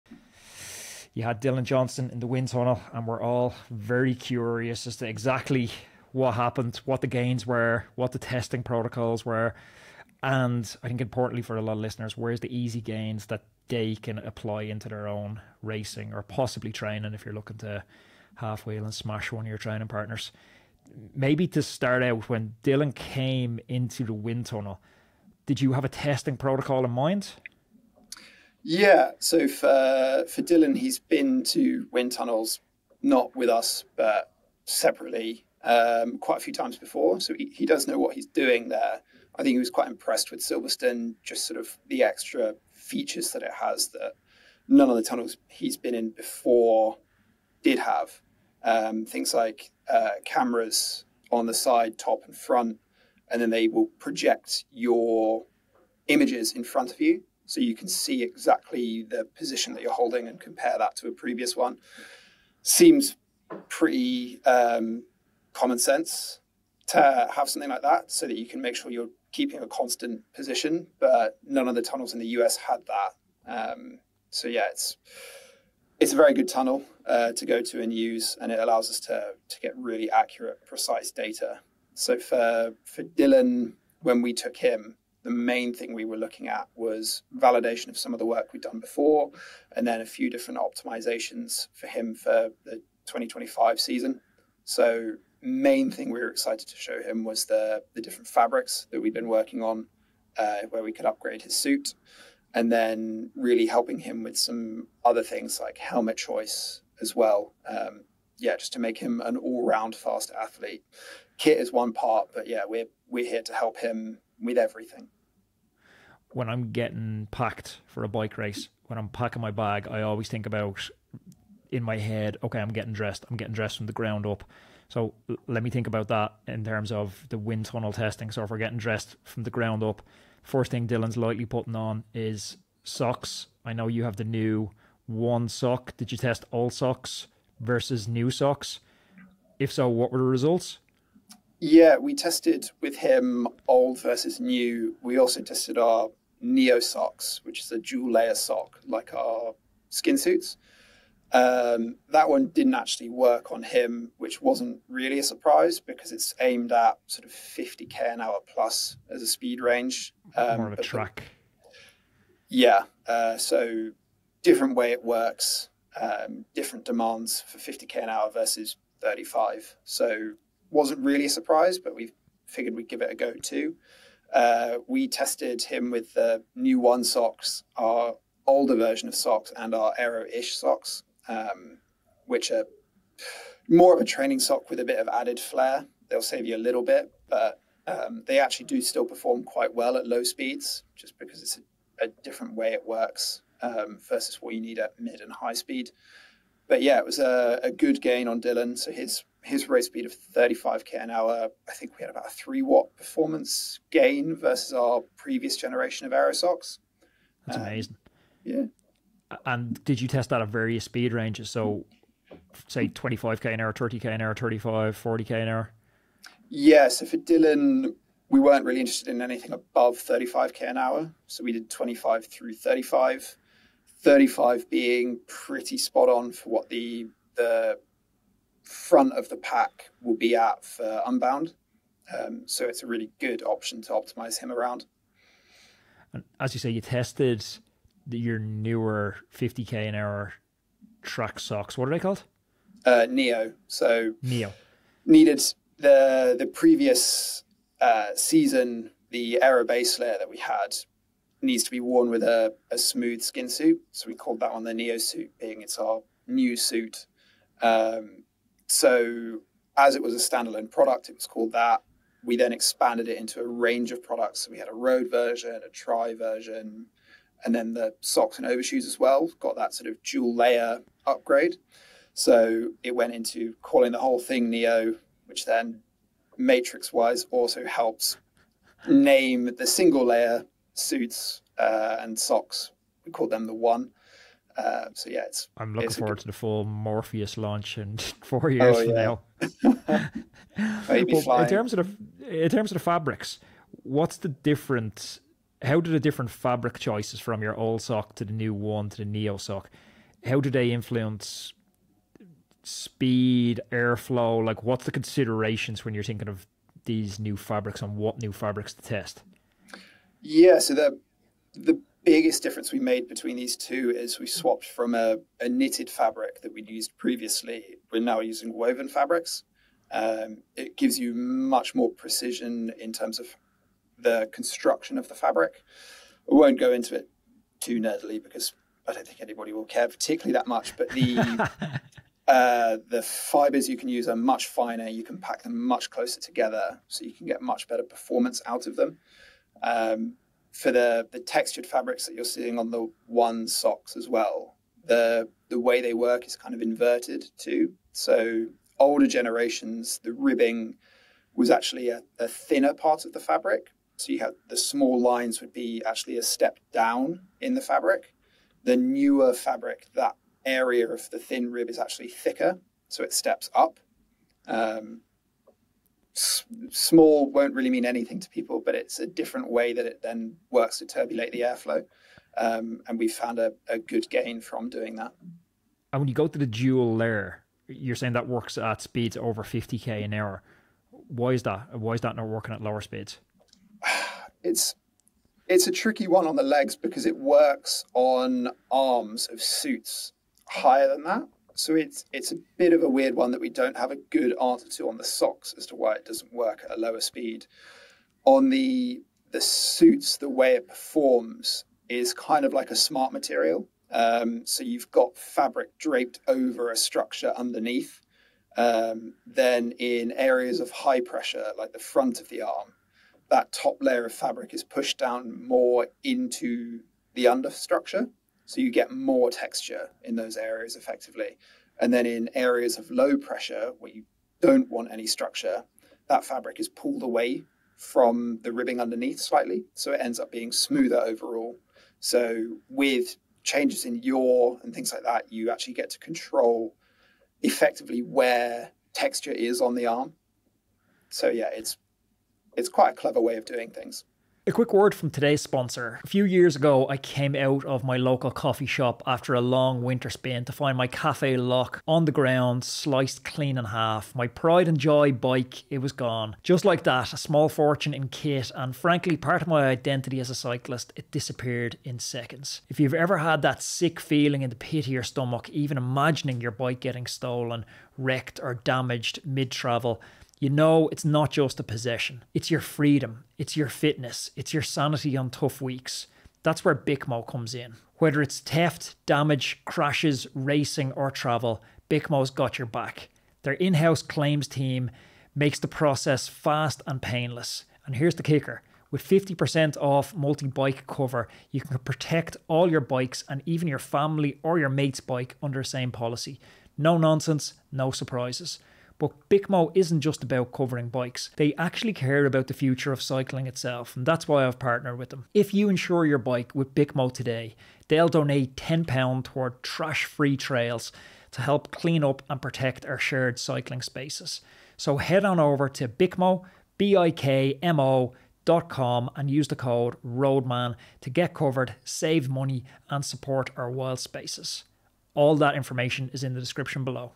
Dylan Johnsons Surprising Wind Tunnel Results Revealed
An extract from full podcast with Sam Calder (Rule 28) where we discuss his aero finding in the wind tunnel with Dylan Johnson